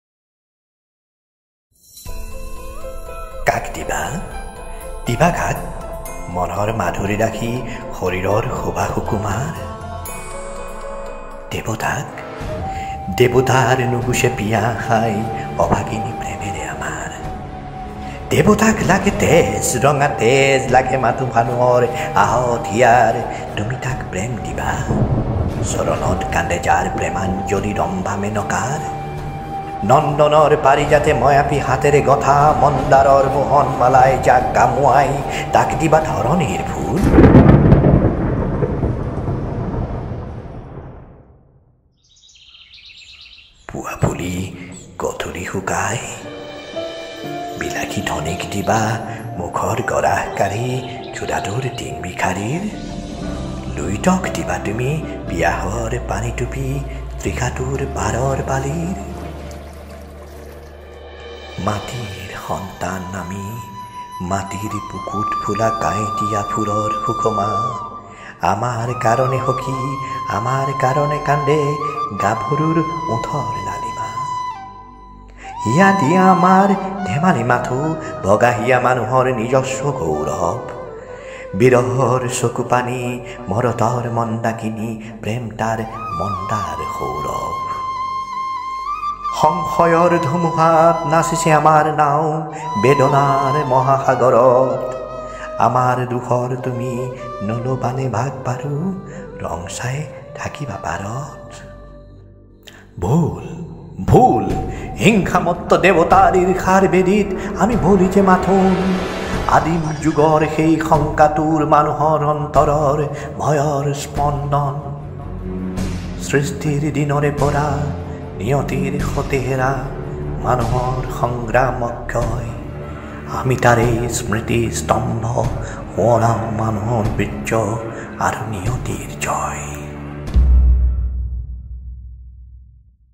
দেকান্ত বৰুৱাৰ “দেৱদাসী”
Recitation by Satyakam Borthakur